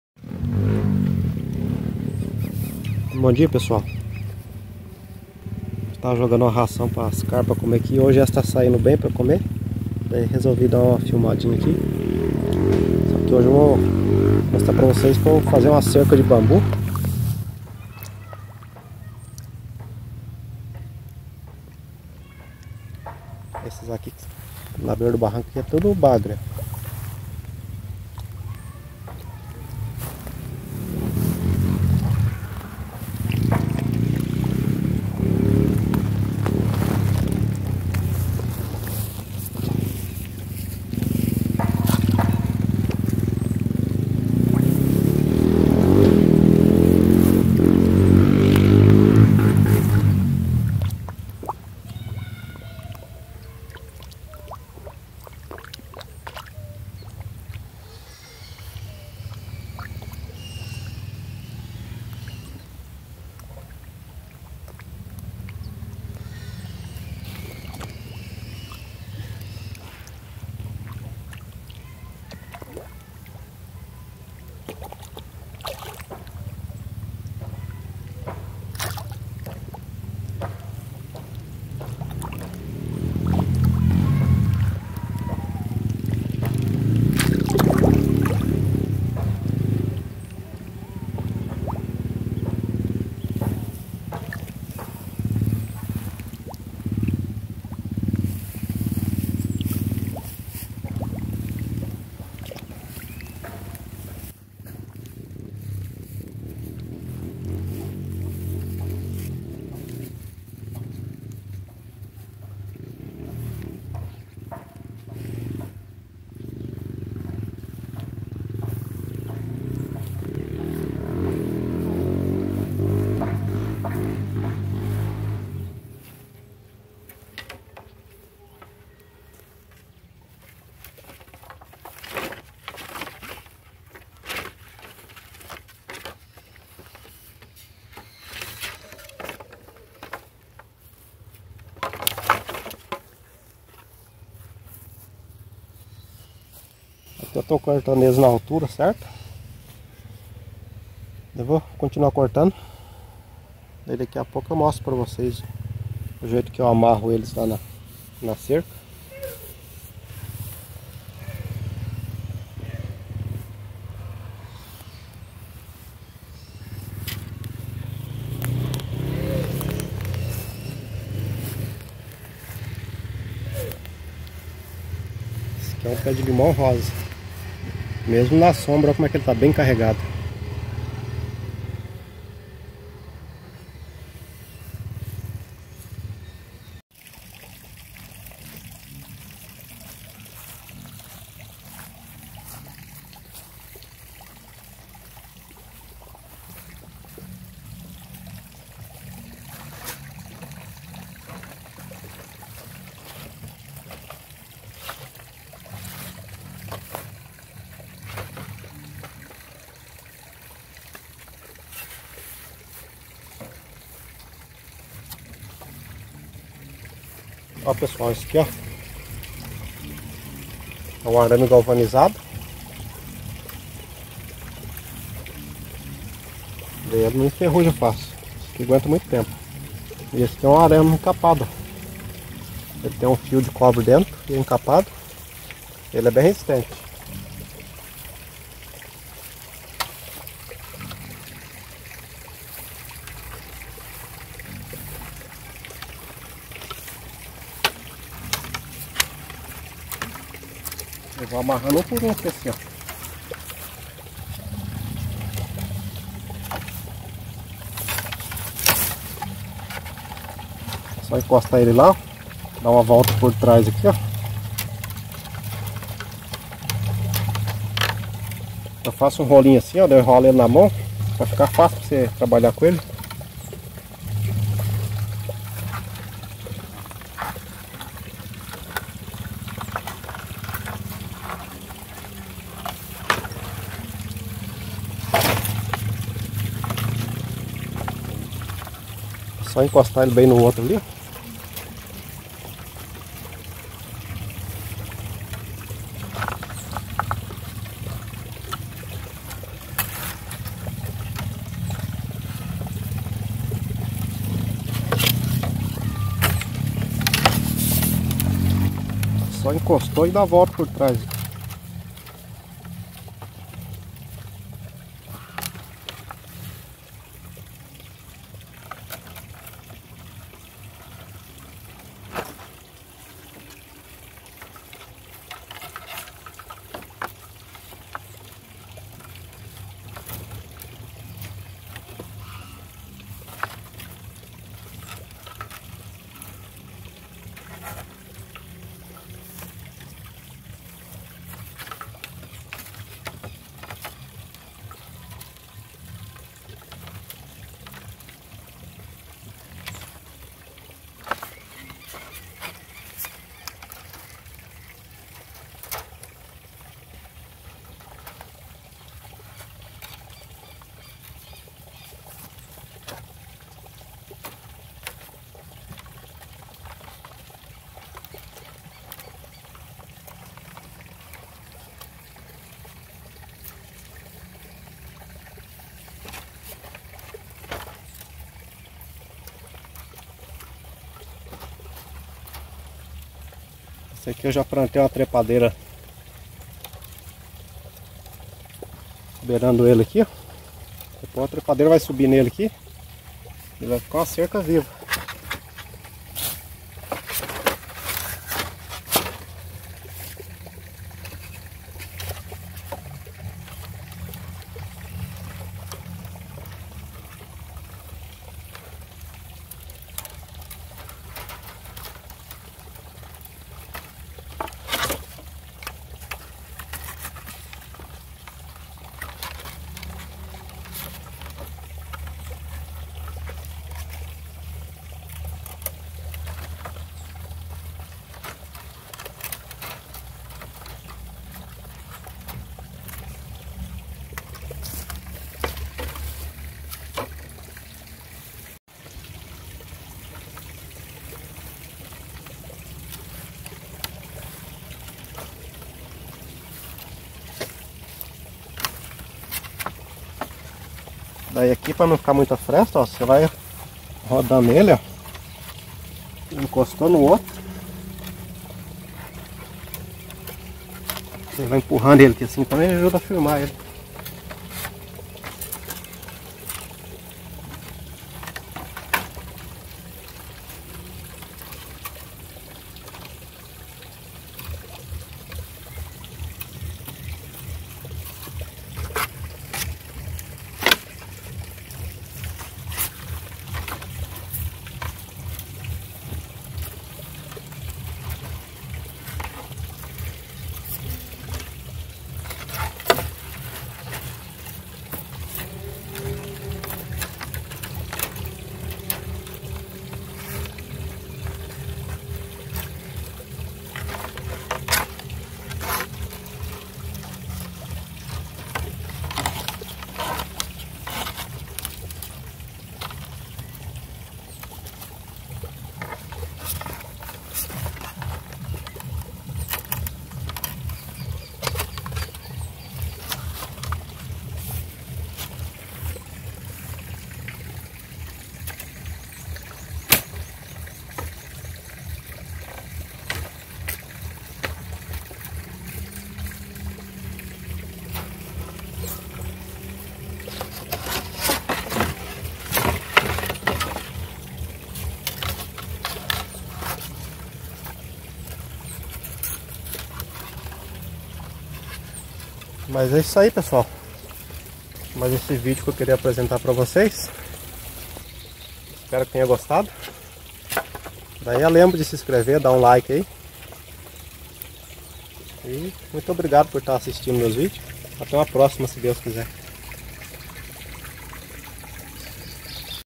CERCA RÚSTICA, BAMBU ORNAMENTAL - Ze Das Palmeiras
Fazendo uma cerca ornamental e cortando os bambus. mostrando as carpas que não apareceram no ultimo vídeo.